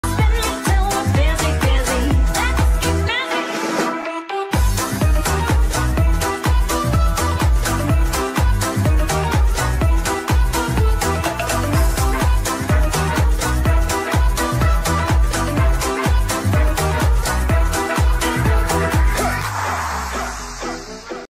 Dizzy dizzy meme // Gacha club // ft epic peeps // pastel orchid
Hi guys!!
I started this before Breezy Riko changed her channel and OC so sorry qwq
Time taken: 4 days
Apps used: CuteCut pro, IbisPaintX, Gacha club, CapCut
🌸🤍 stay safe! 🤍🌸